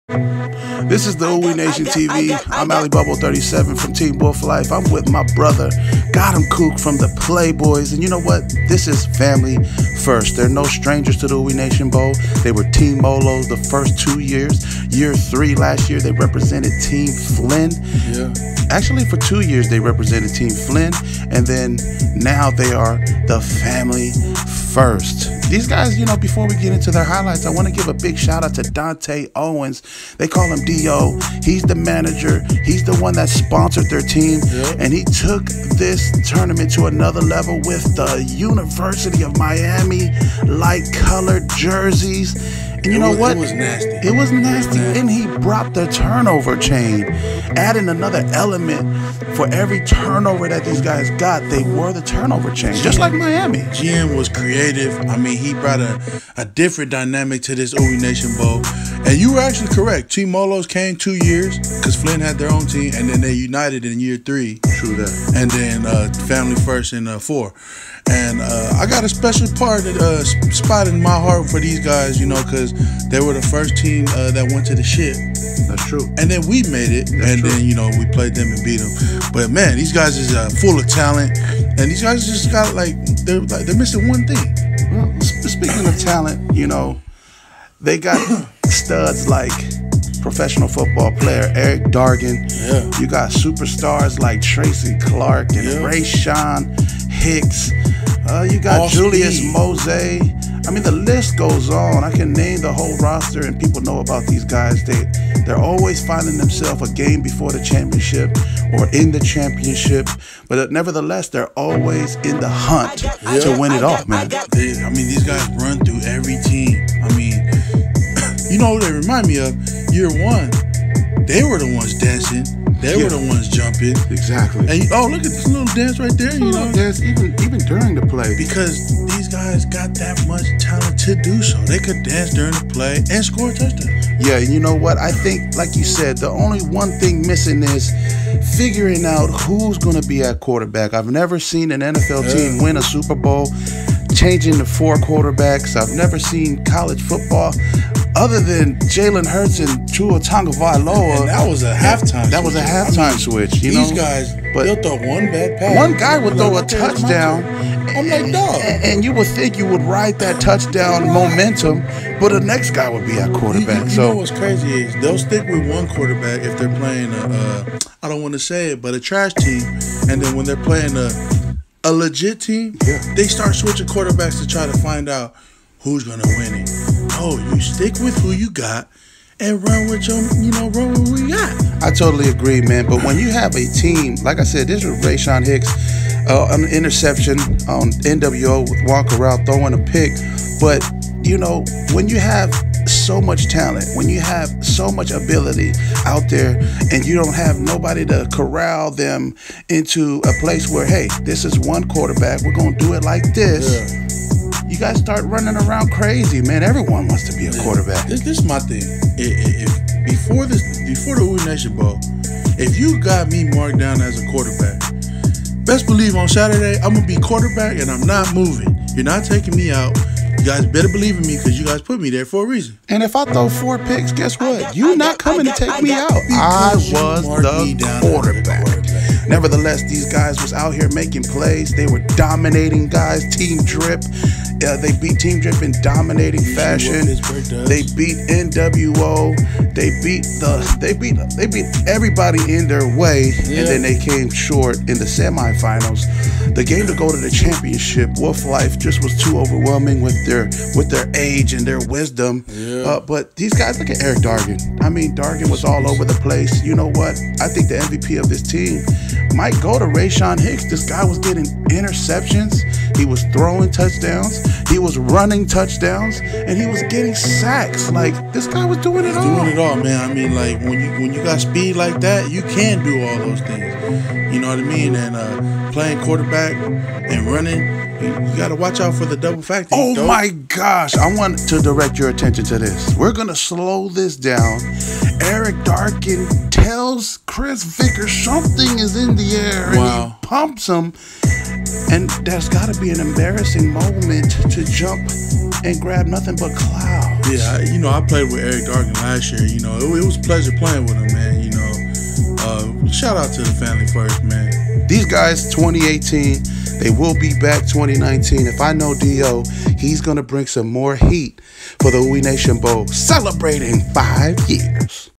FAMILY FIRST OoweeNationBowl IV
A competitive flag football tournament in memory of Alex Tui. Raising funds towards the AMT Scholarship honoring Pastor Alex Toeaina Sr. for 30+ years in the community, helping youth and young adults to a better way.